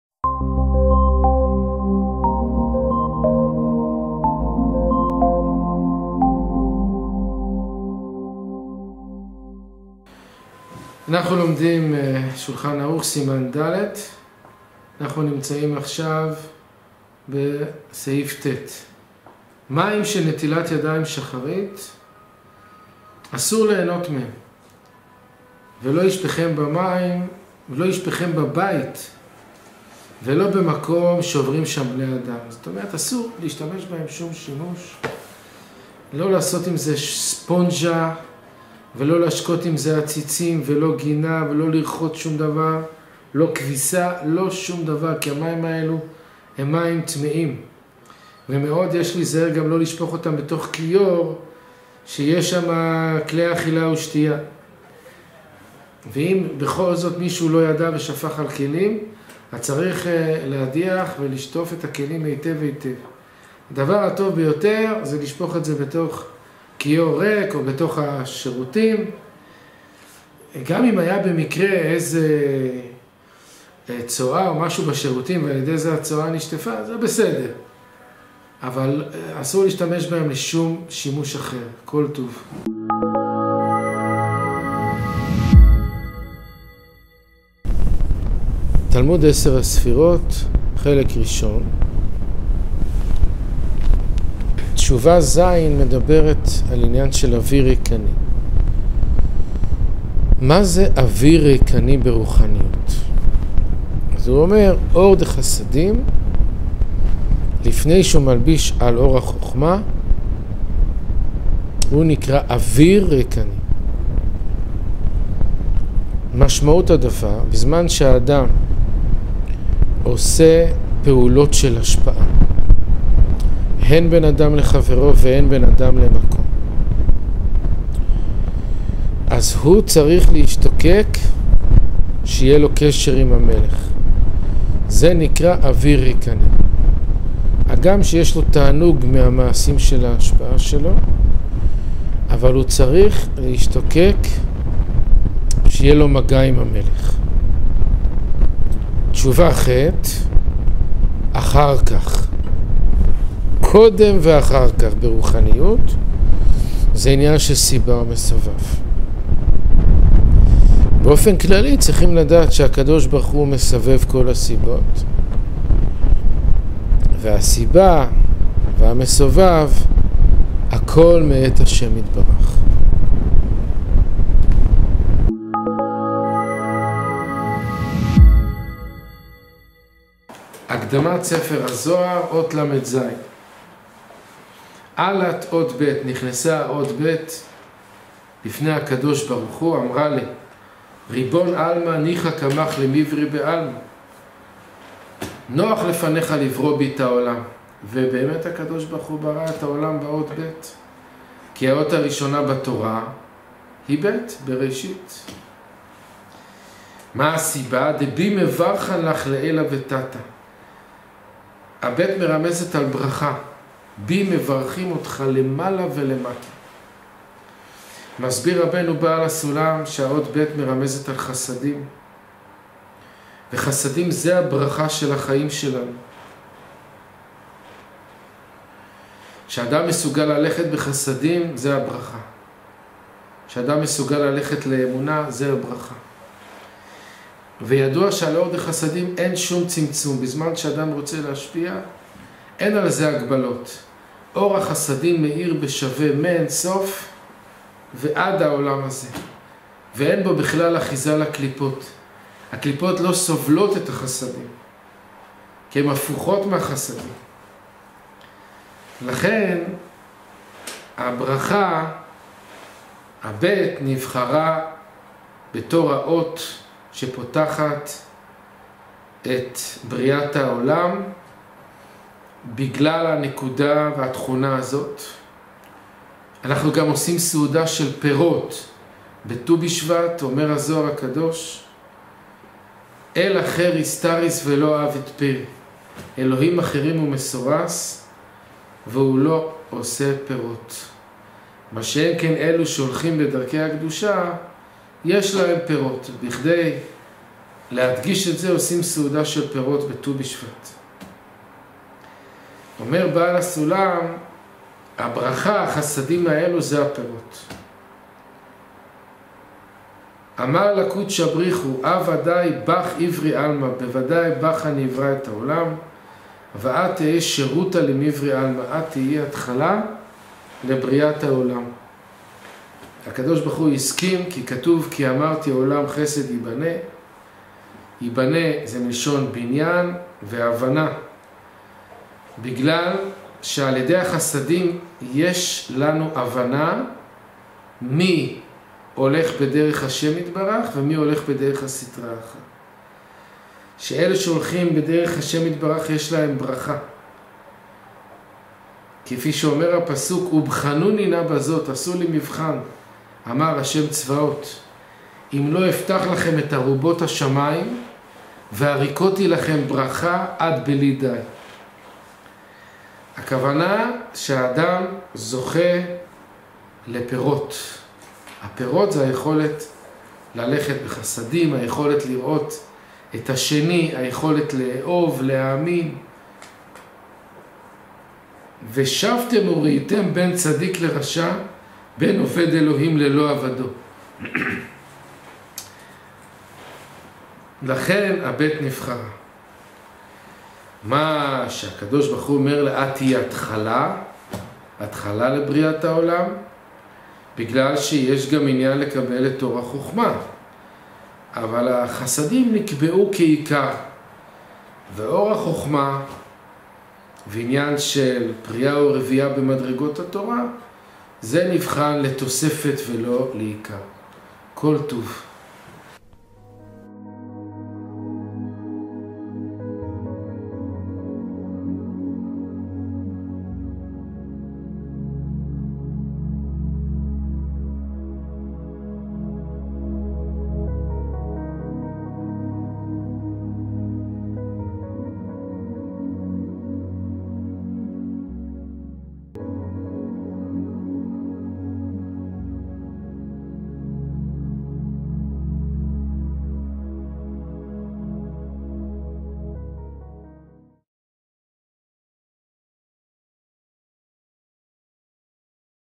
הלימוד היומי #37 - הלכה, זוהר ותע"ס ב- 15 דקות,ז' שבט התשע"ט, 13-1-2019
האדמו"ר רבי אברהם מרדכי גוטליב שליט"א
הלכה - שו"ע, או"ח, דיני נטילת ידים, סימן ד' סעיף ט'
תע"ס - חלק א', לוח התשובות לפירוש המילות, תשובה ז' ח'
הזוהר הקדוש - הקדמת ספר הזוהר מאמר אותיות דרב המנונא סבא אות לז'
להצטרפות לקבוצת וואטסאפ לצורך קבלת עדכונים, הקליקו על הלינק הבא:
ברכת שלום
- בית מדרש ללימודי קבלה ופנימיות התורה בדרך אשלג בראשות הרב גוטליב